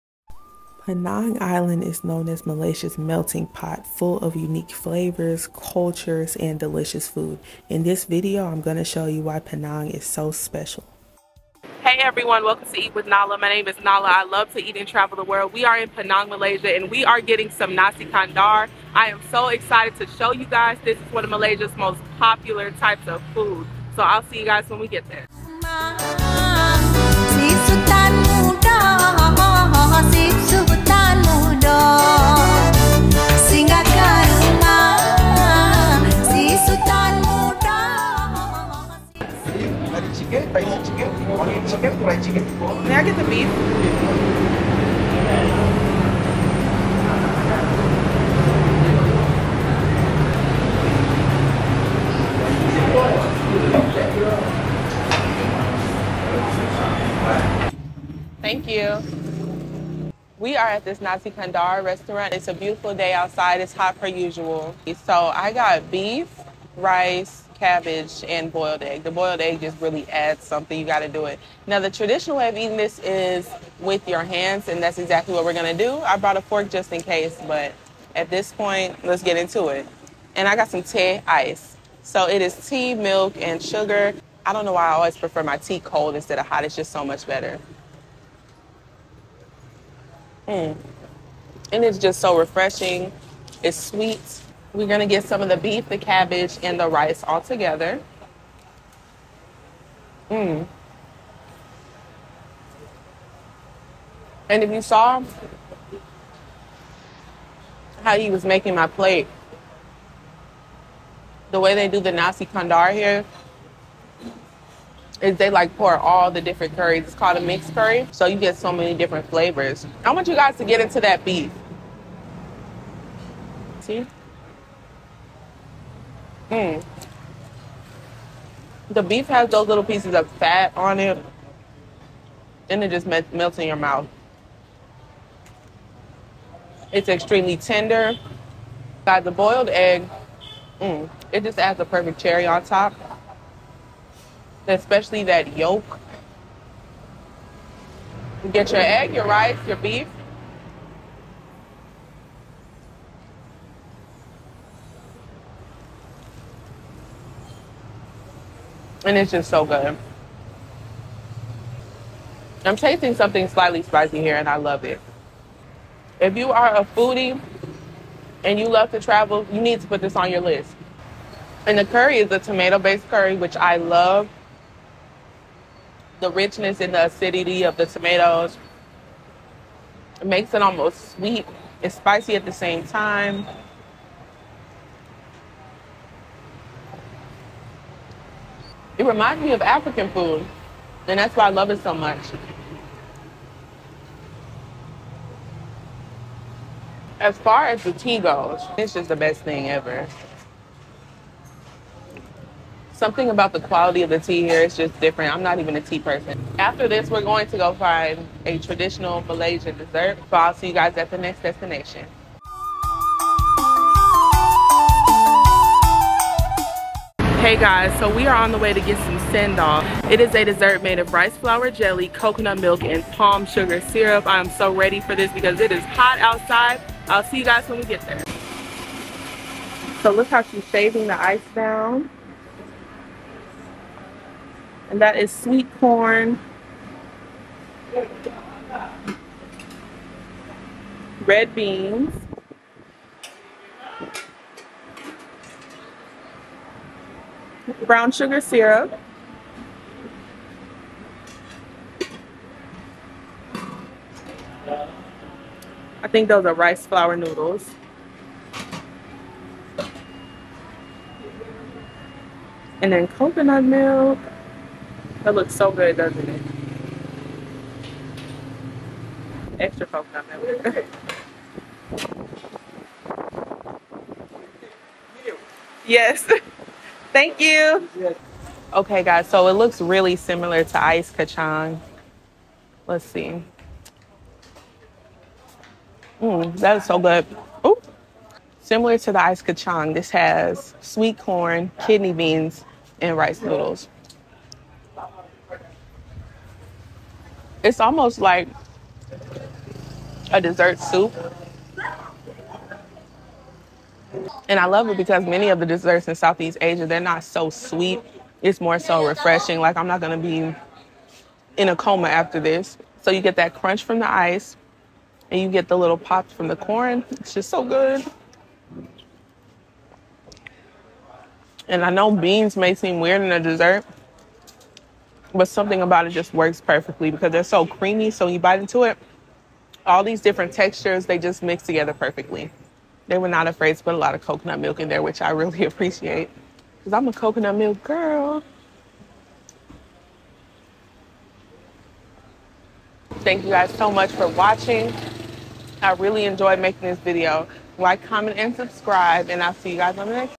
NASI KANDAR & CENDOL: MALAYSIA’S MOST-LOVED DISHES
Hey everyone ❤️ Join me as we dive into one of Malaysia’s most-loved dishes, Nasi Kandar. It is one of my personal flavors due to rich, delectable flavor profile. In this video, we also try another Malaysian favorite, Cendol. I can’t wait to get into these flavors with you!